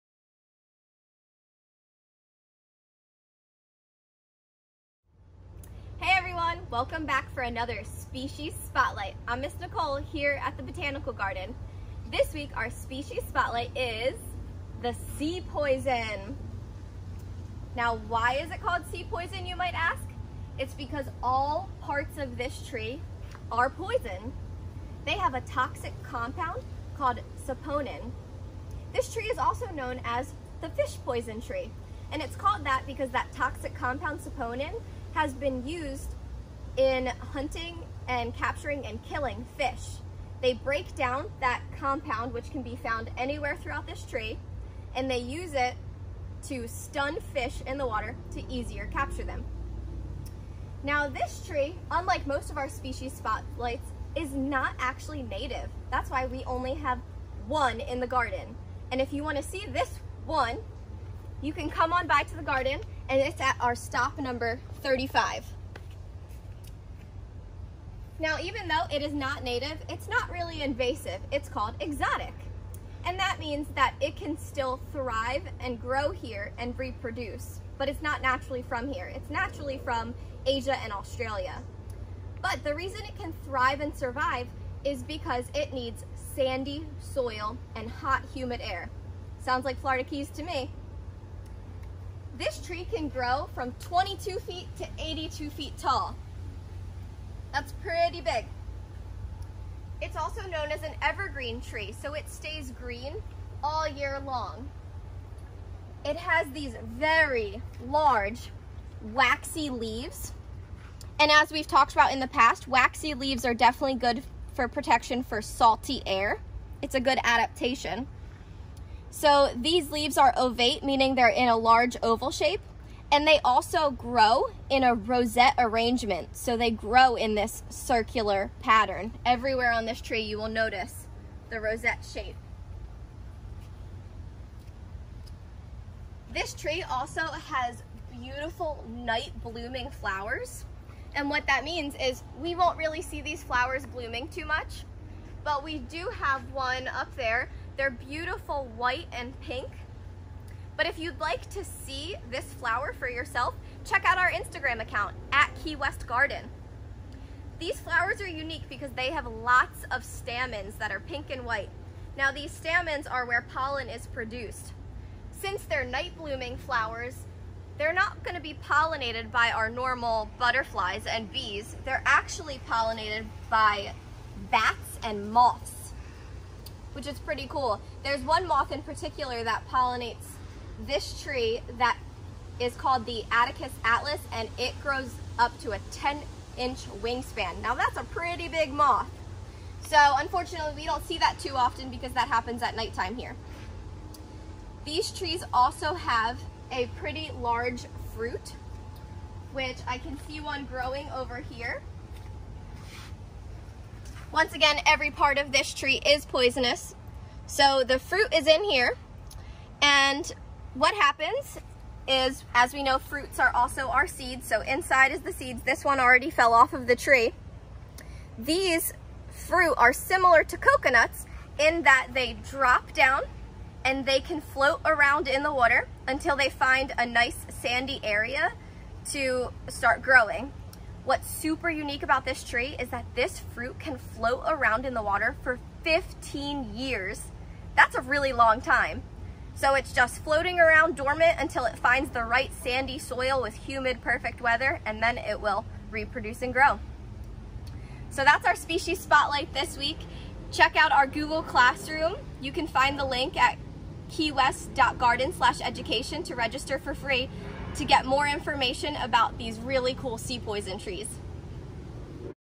Species Spotlight: Sea Poison
This week’s species spotlight is the Sea Poison tree! All parts of this species is poisonous!

Check out KWVirtualGarden by registering for free at Keywest.garden/education for more about this species!